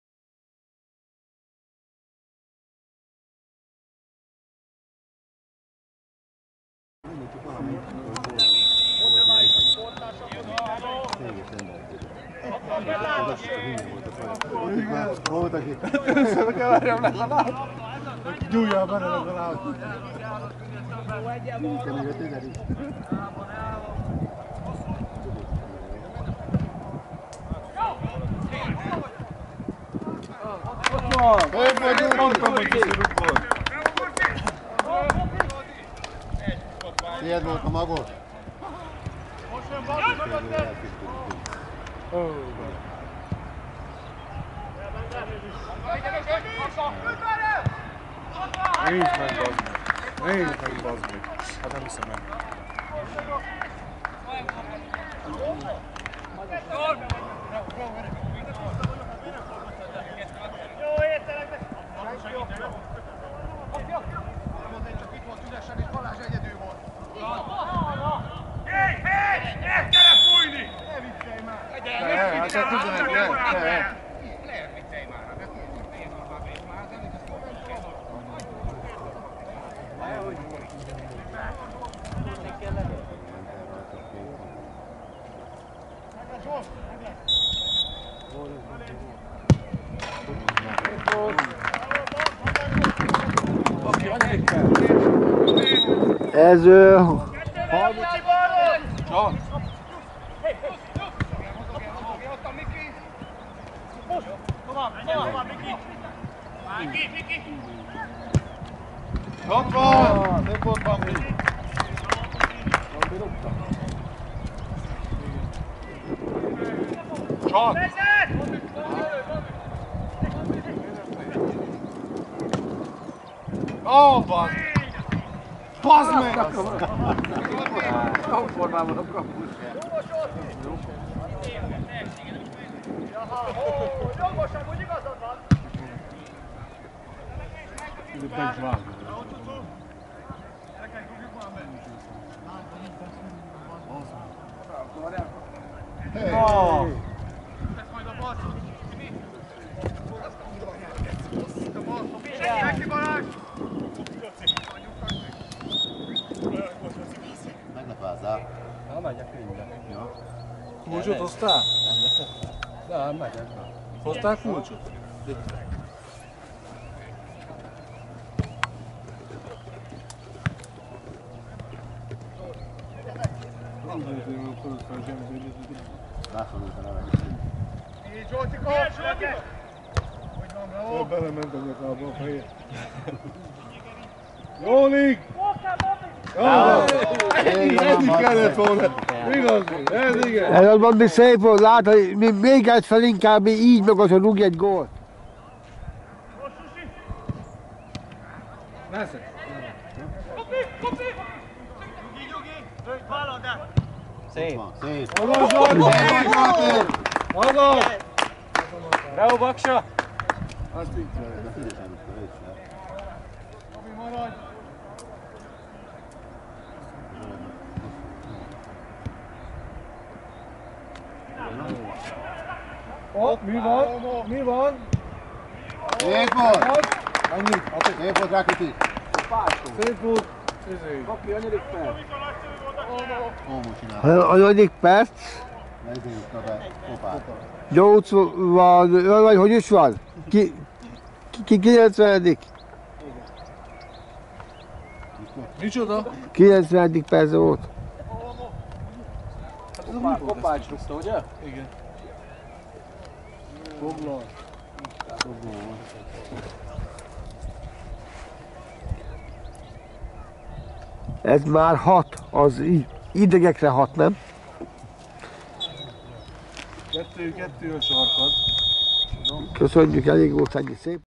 Kétegyházi SE - Okány KSK gólösszefoglaló /2021.04.24./
Végeredmény: 1-6
Operatőr: Bak László
Szerkesztő, vágó: Varju Richárd